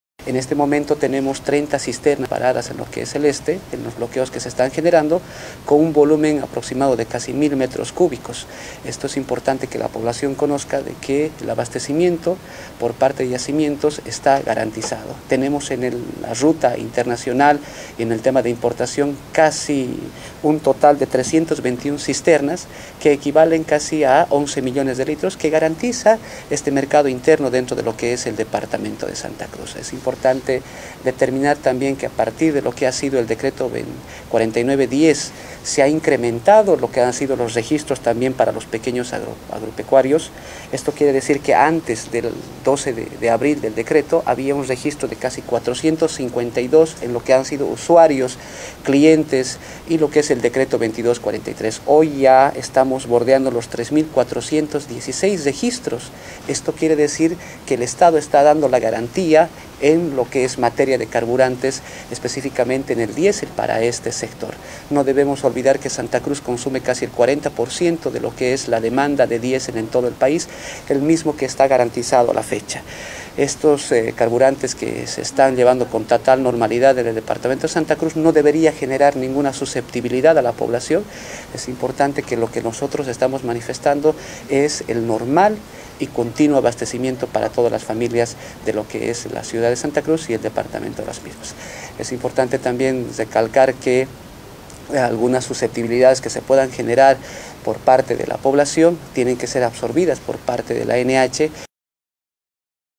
Director Nacional ANH garantiza combustibles en Santa Cruz pese a bloqueos en algunas rutasGermán J
Director Nacional ANH garantiza combustibles en Santa Cruz pese a bloqueos en algunas rutas

Germán Jimenez Director de la Agencia Nacional de Hidrocarburos(ANH) garantizó el abastecimiento de combustible en Santa Cruz, ante los reclamos de escasez debido a los bloqueos de carreteras principalmente en la carretera al Beni y en la zona norte del Departamento. Jimenez, pidió no generar ninguna susceptibilidad al sector productivo y negó qué se esté racionalizando el combustible en el departamento qué consume el 40% del total del país. Agregó, que hay 30 cisternas varados en los bloqueos y señaló qué hay unos 11 millones de litros de combustibles para garantizar el abastecimiento.